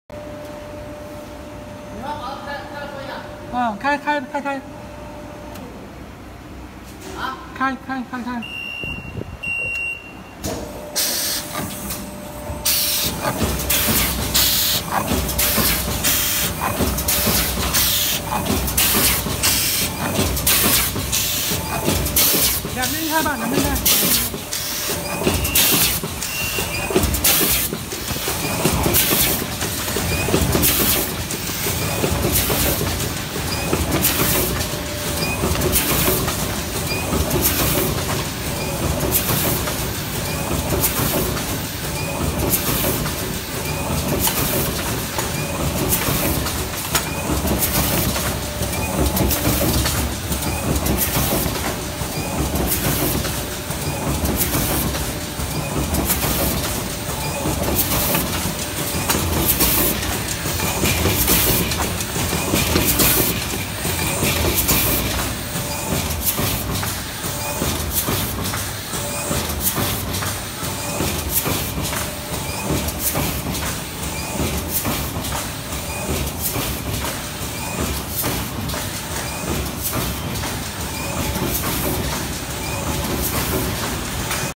hydraulic double station paper tray machine test rectangle & round tray 700GSM for Saudi client
hydraulic double working station paper tray machine test rectangle & round tray 700GSM for Saudi client

Ruian Main Resource Paper Product Factory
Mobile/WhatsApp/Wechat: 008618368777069
Address: No. 388 Sun Ao Road Feiyun Street Ruian City Zhejiang Province - 325207 China